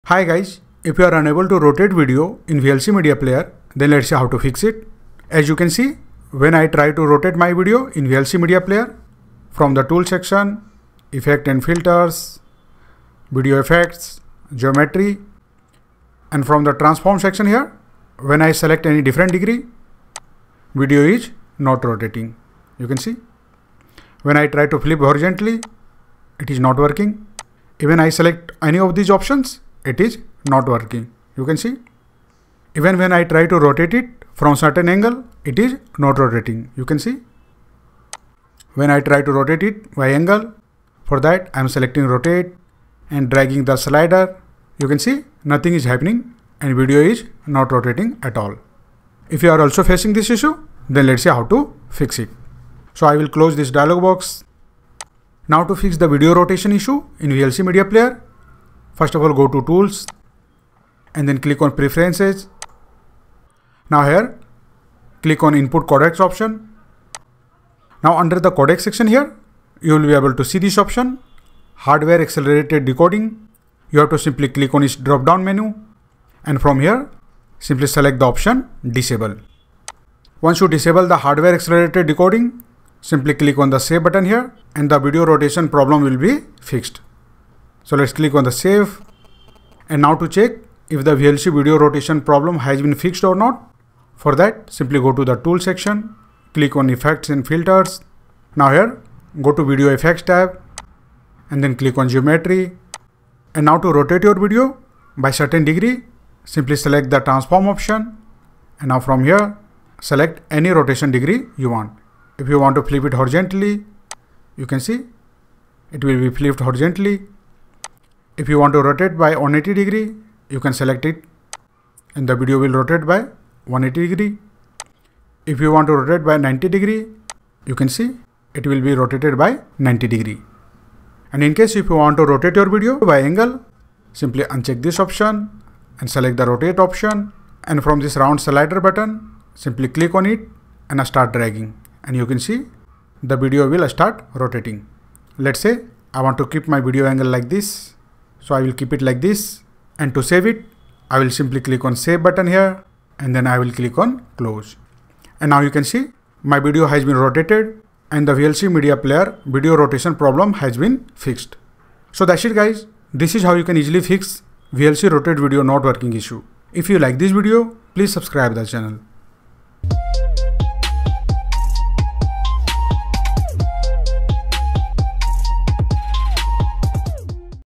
VLC Rotate Video Not Working Fix | How to Rotate Video in VLC Media Player
Learn how to fix VLC rotate video not working issue and enjoy your videos correctly oriented. Follow these quick steps to know how to rotate video in VLC Media Player without hassle